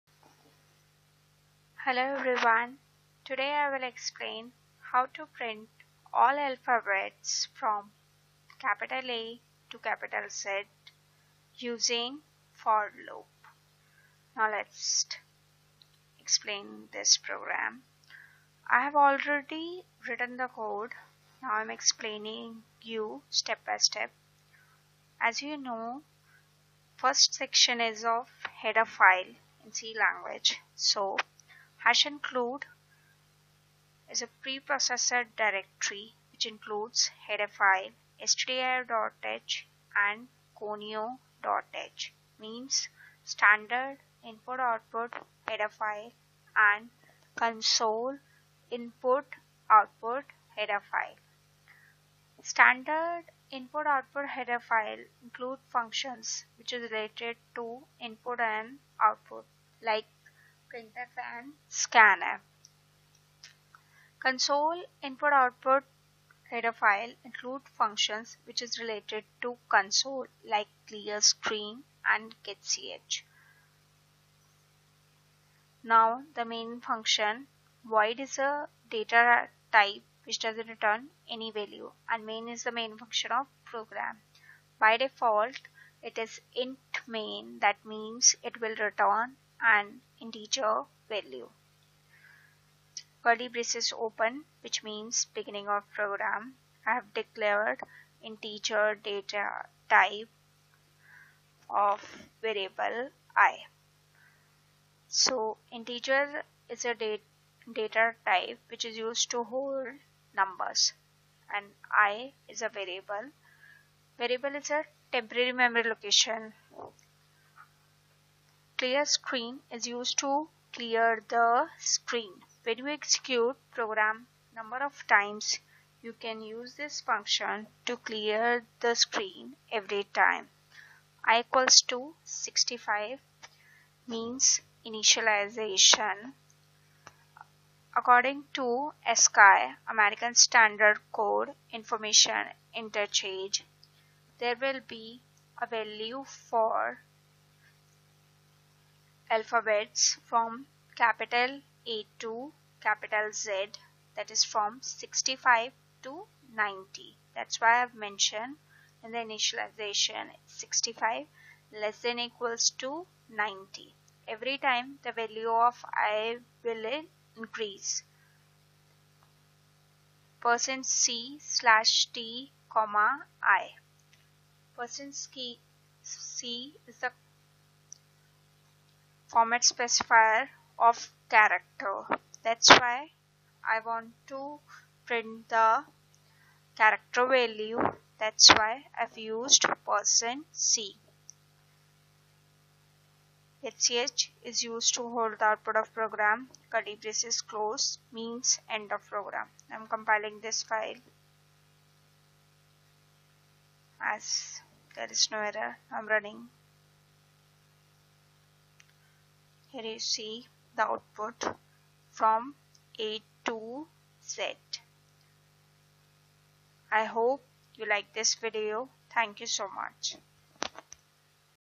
Program to print all alphabet from A to Z in C Language
A well explained program to print all alphabet from A to Z in C Language .I hope you like this video